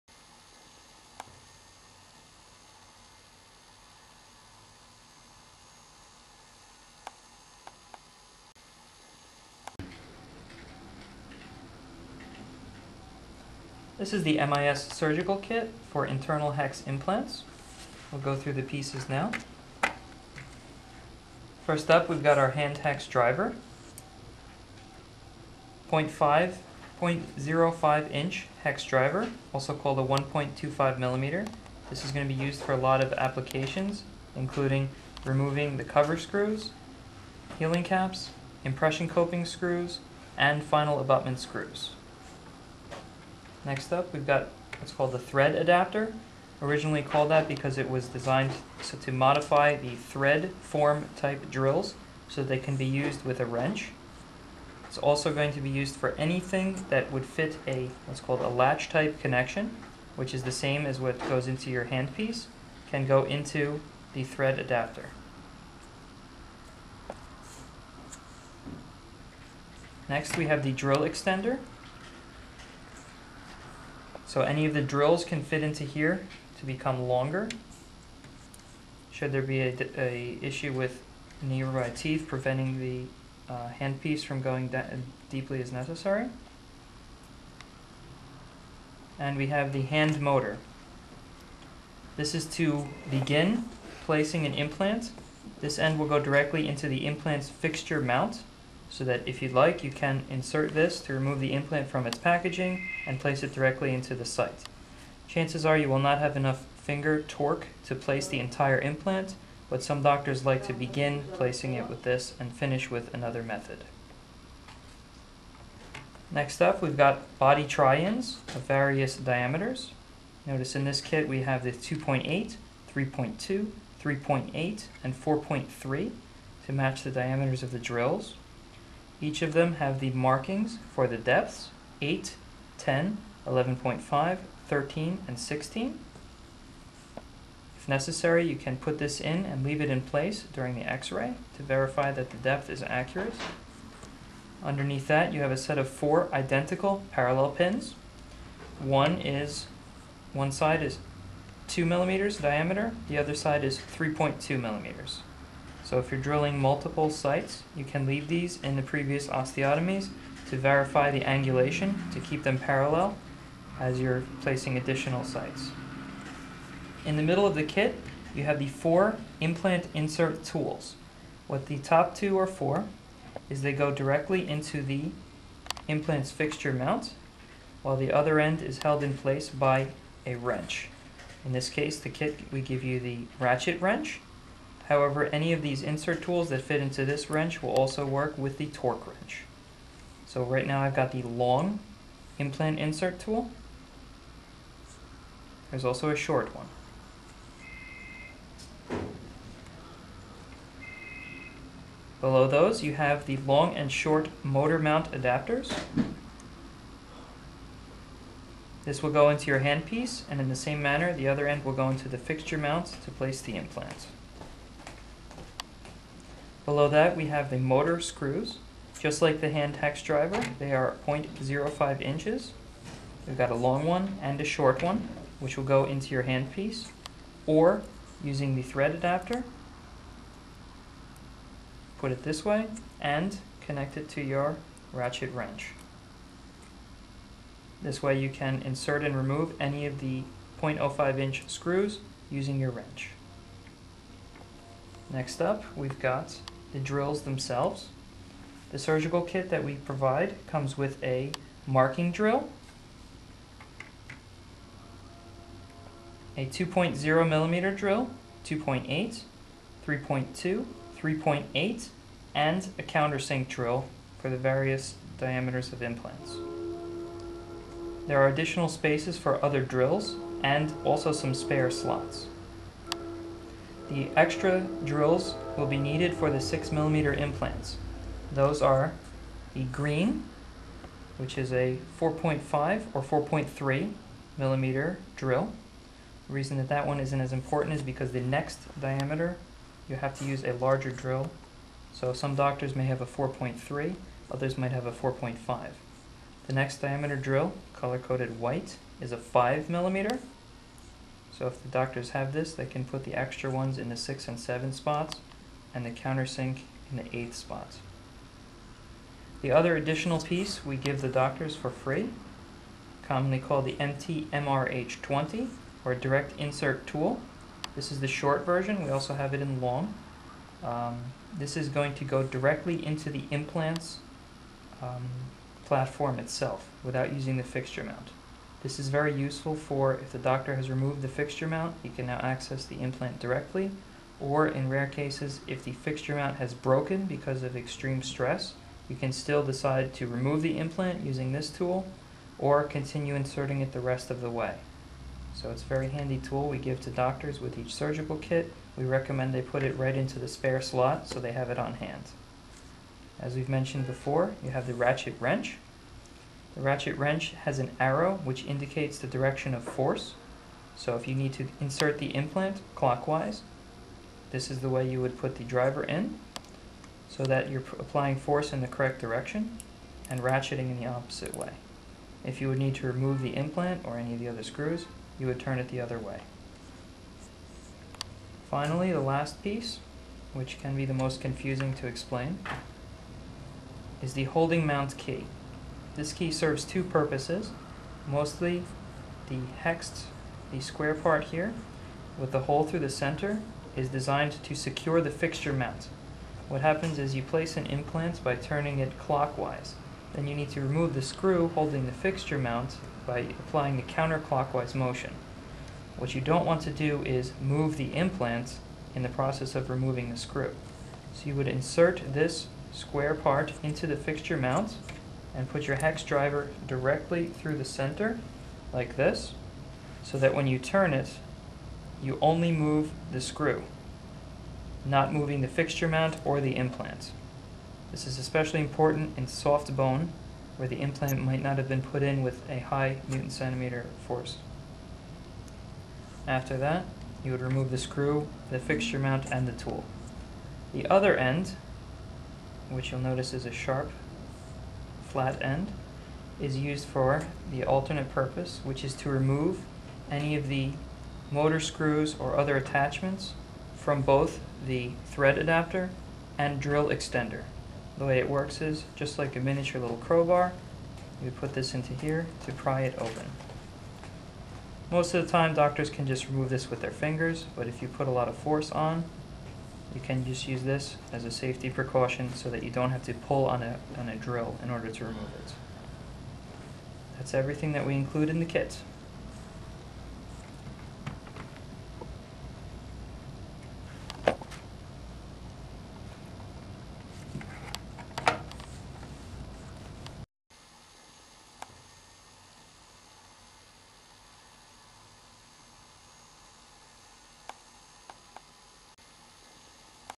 Dental implant tools
A training video showing the MIS-implants surgical kit